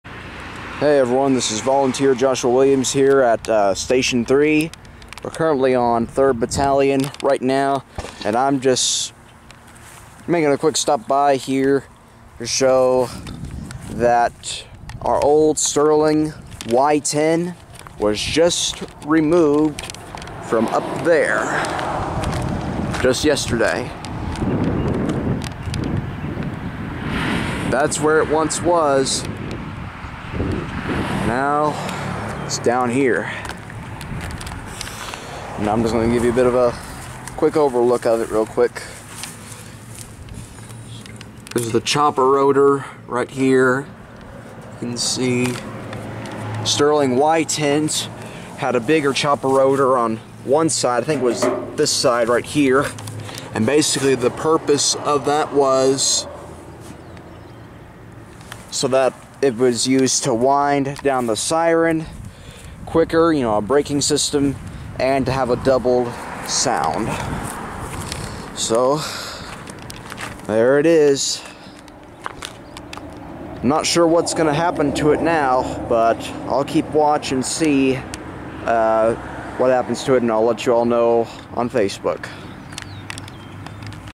Old Station 3 Sterling Y-10 Siren Removed 4-13-2019
We just removed this old Sterling Y-10 yesterday (same one I talked about years ago). We don't know exactly what is going to happen to it at this time. I'm checking in with everyone at the department to see what may happen to it for all siren enthusiasts. This siren is currently located on 4130 Camargo Road, Mt Sterling, KY 40353. There's been some talk so far to possibly refurbish the siren and use it for weather purposes. And I myself have made some suggestions to have this siren as an addition to our Fire Chief's Fire Museum he is making for the department. It may be some time however before an update is released.